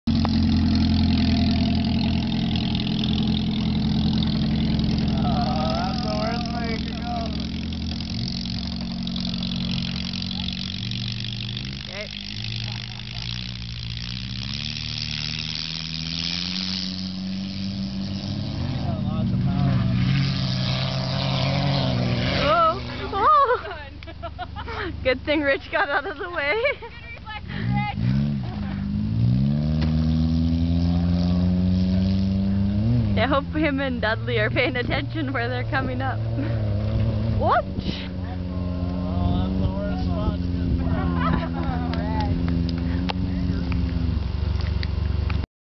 Dirt Car Hill Climb at Big Kooly Raceway
The Flying Pigeon can't make it up the hill so it bails out. Viagra makes it over the top.